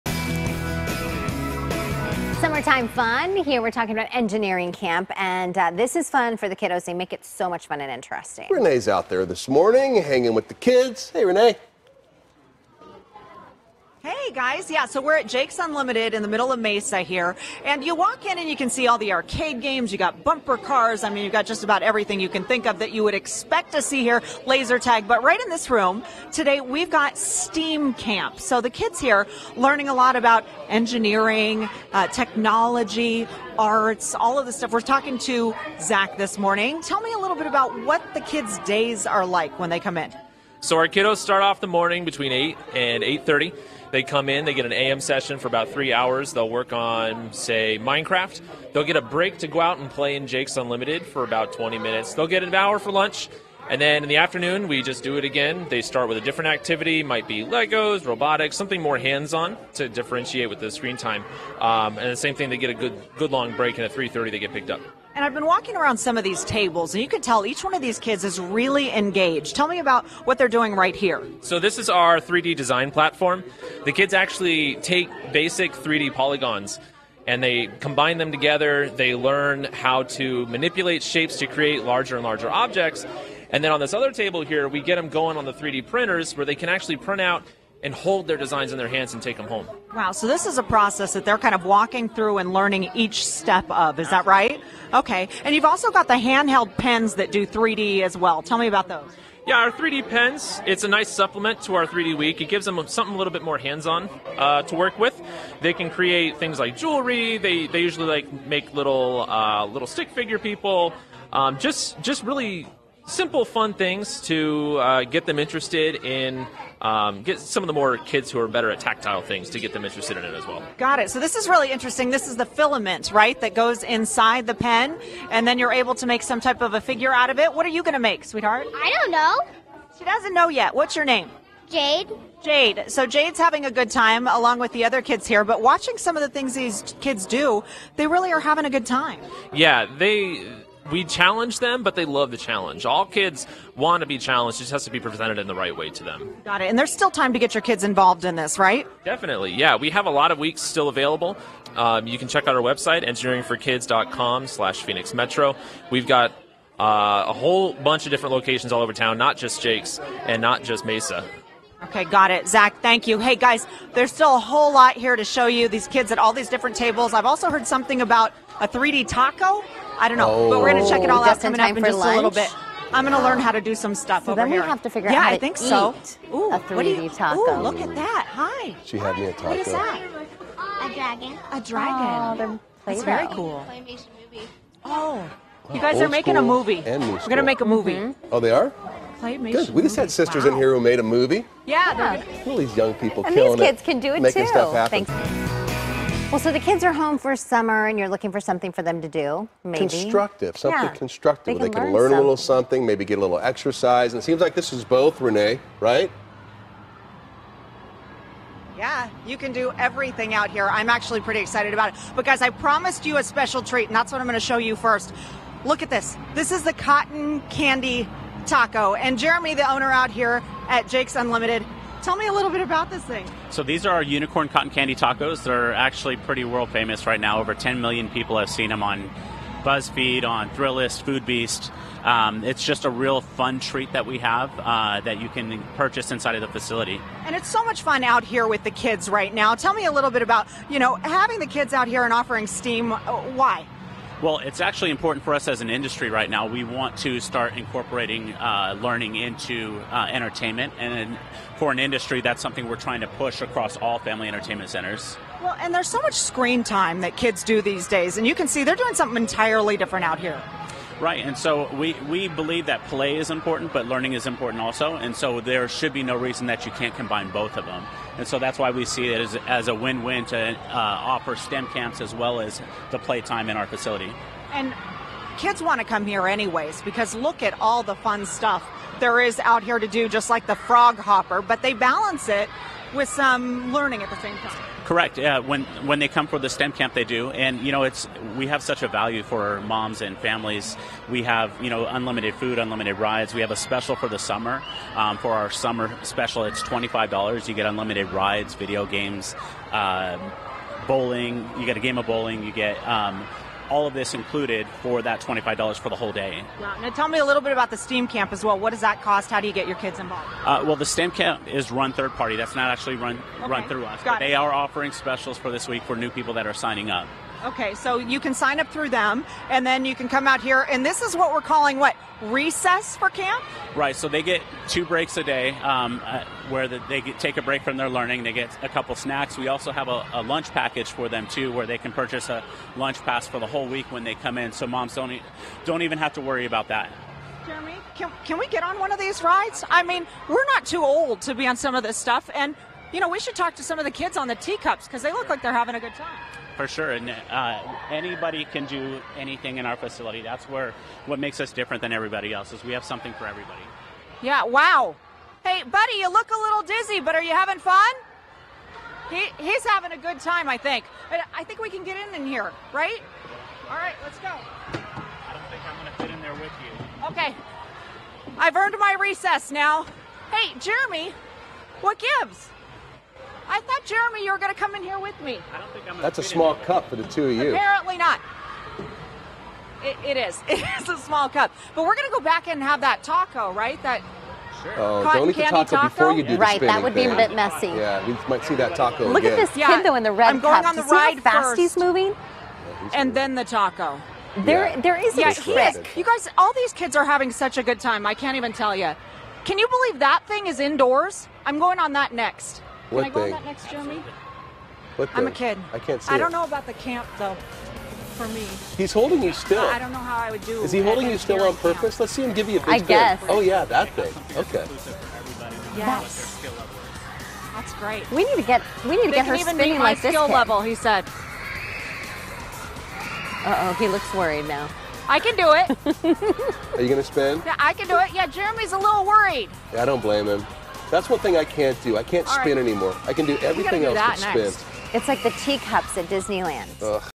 Summertime Fun: Jake's Unlimited engineering camp
Fox 10's Renee Nelson checks out the engineering camp offered this summer at Jake's Unlimited in Mesa.